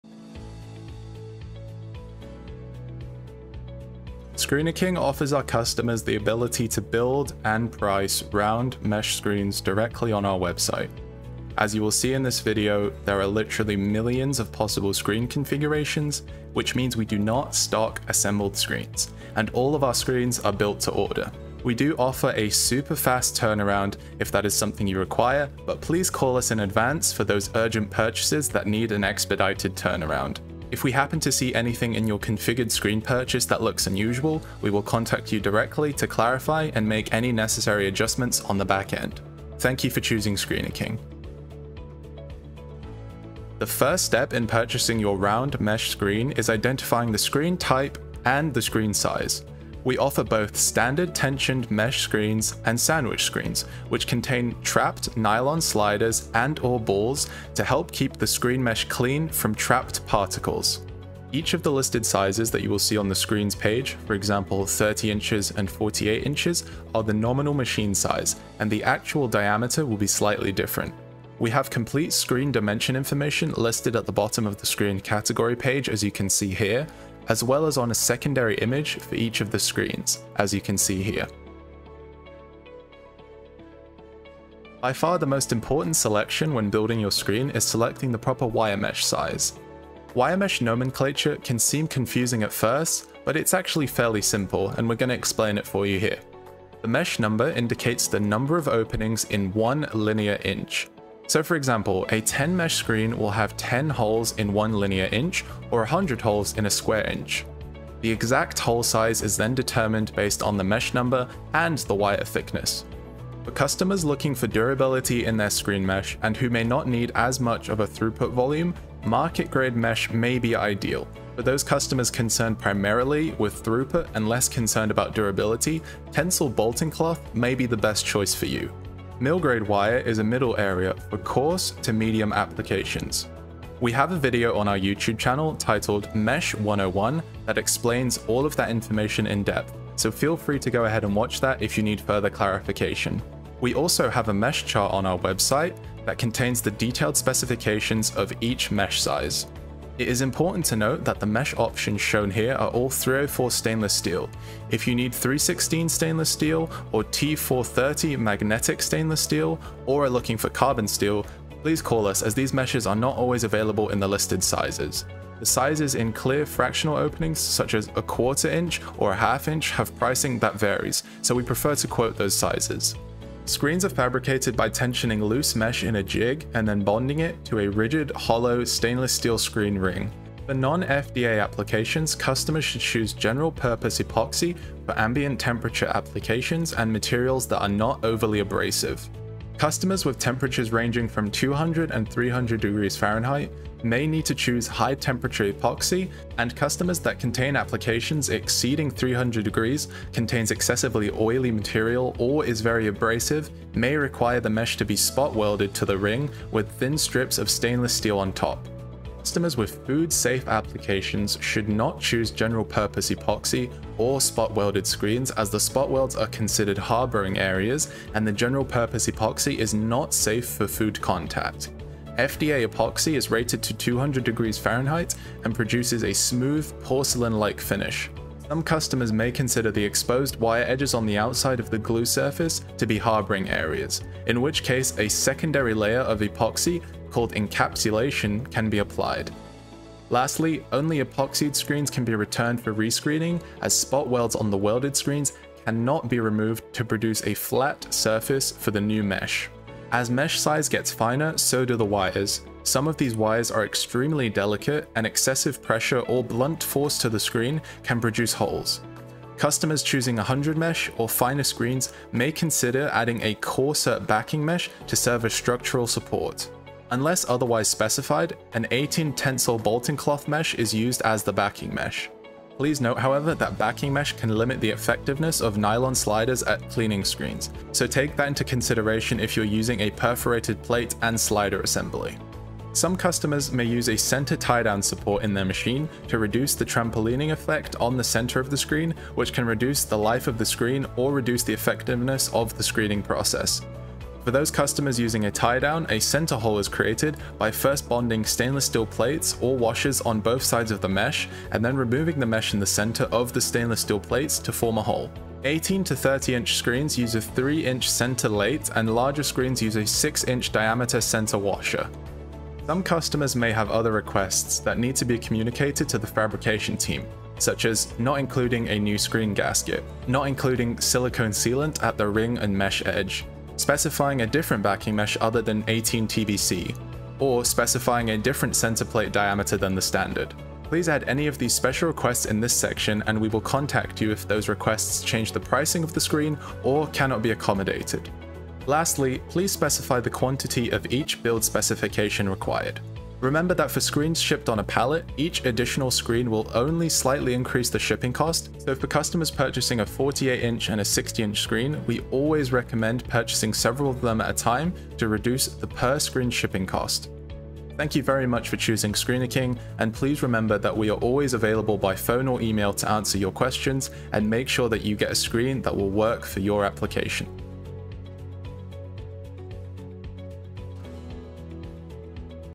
How To Buy A Round Vibratory Mesh Screen | ScreenerKing®
ScreenerKing supplies complete industrial separation units, screens, and replacement parts for Sweco, Kason, and other common vibratory screeners.

This video is a complete overview of how to spec and build out a round vibratory mesh screen through our custom screen building on our website.

Chapters:

0:00 Introduction
0:41 Section 1 - Choosing A Screen Type and Size
1:23 Section 2 - Selecting The Screen Specifications For A Chosen Diameter Screen

to build and buy your custom screening unit and screens today!